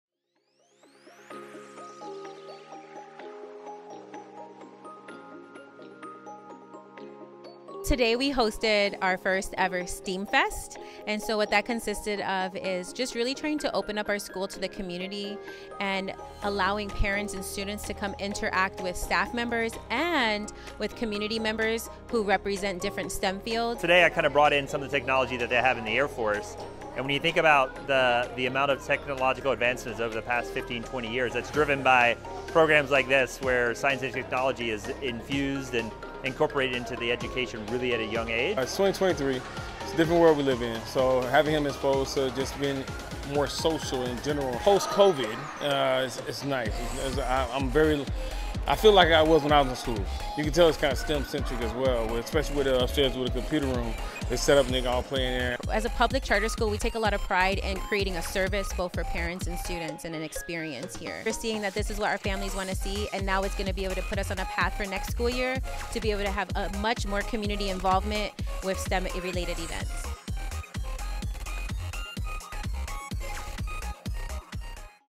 SST Northwest S.T.E.A.M. Fest
SST Northwest held its first ever S.T.E.A.M. Fest, allowing the general public to visit the school to showcase STEM related events.